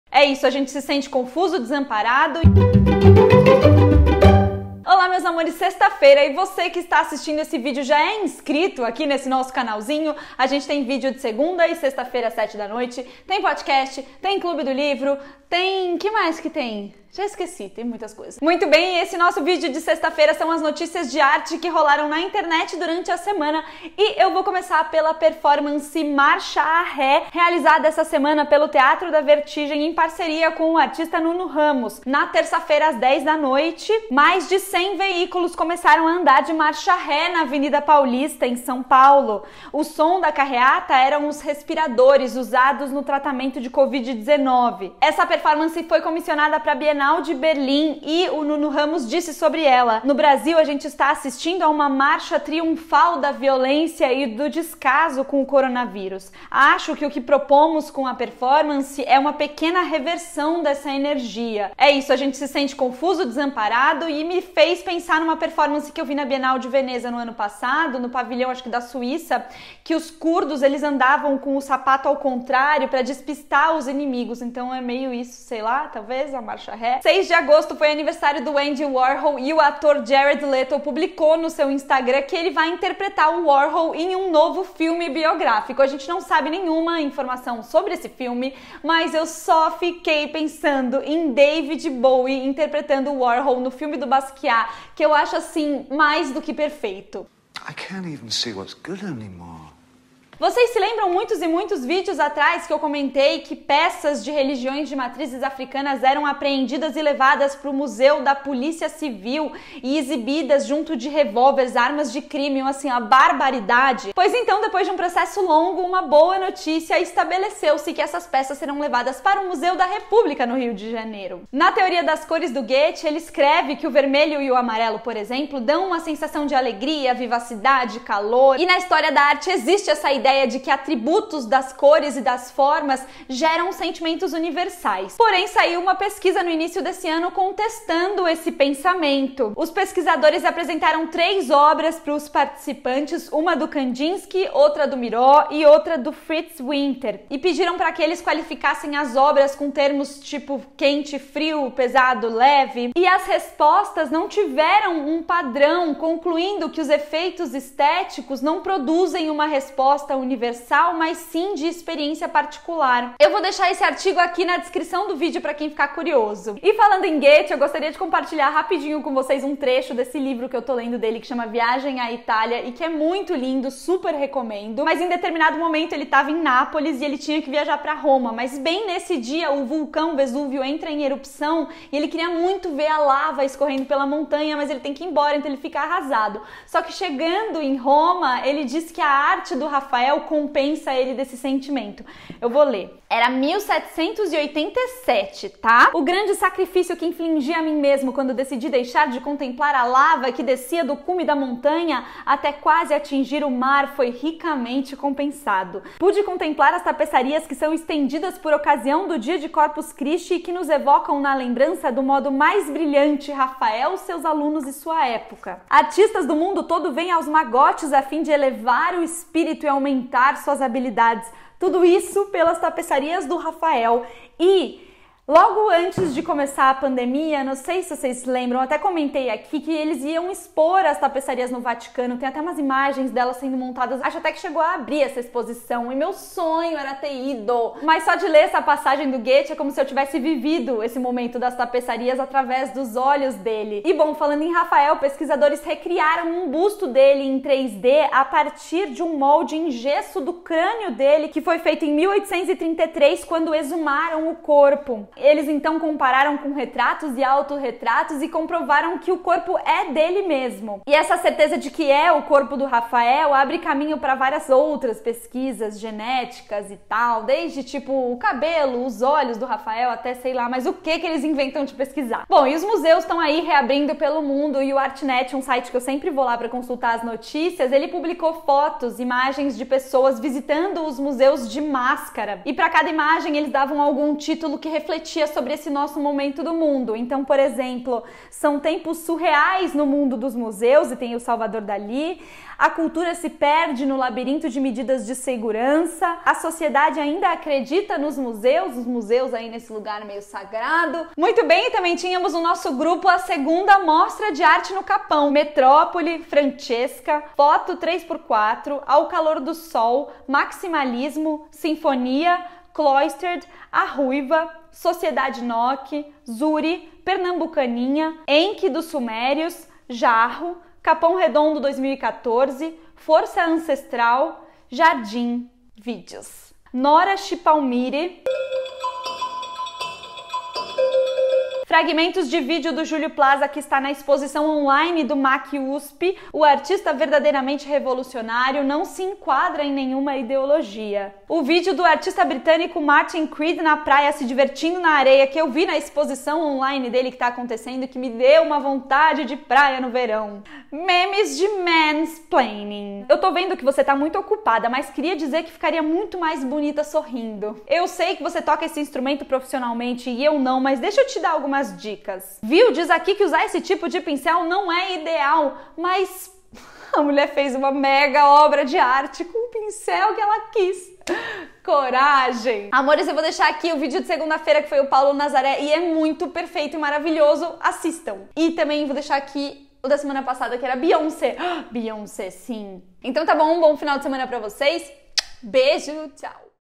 Performance com carreata em marcha a ré na Paulista – Vivi Arte News
Notícias: performance de Nuno Ramos e Teatro da Vertigem coloca 100 carros na avenida Paulista andando de ré para falar do descaso com a covid1-9, Andy Warhol será interpretado por Jared Leto em novo filme e eu só consegui pensar no David Bowie, peças de religiões de matrizes africanas irão para o Museu da República, sentimentos das cores e formas não são universais segundo pesquisa, Rafael Sanzio inspirou Goethe. E mais vídeos e memes.

O curso de introdução à história da arte com o Prof. Rodrigo Naves está disponível neste

Compre os livros que aparecem no canal